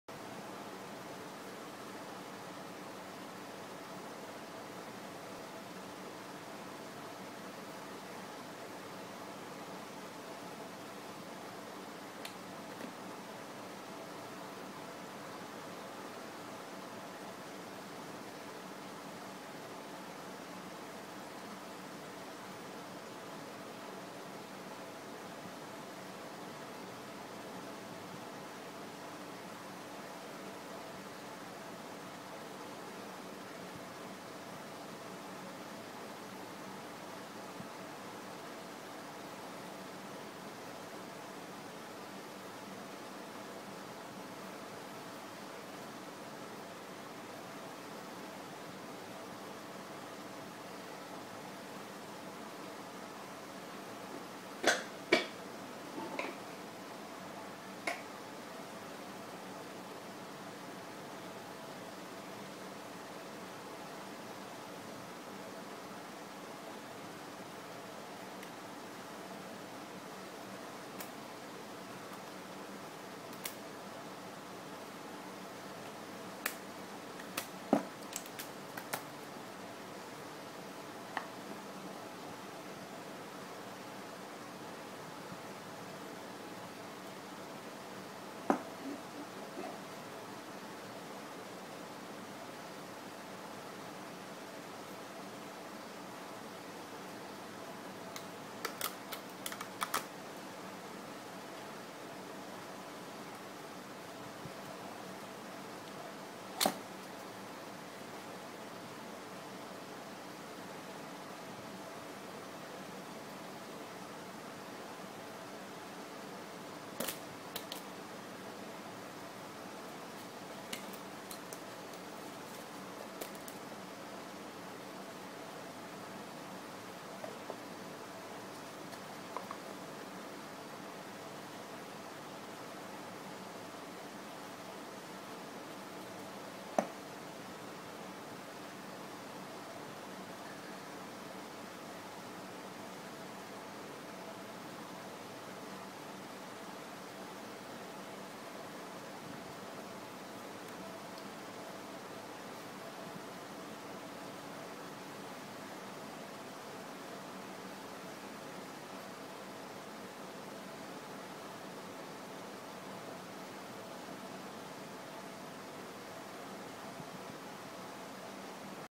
Out of the Box
Easy French drop with fun prop driven finale.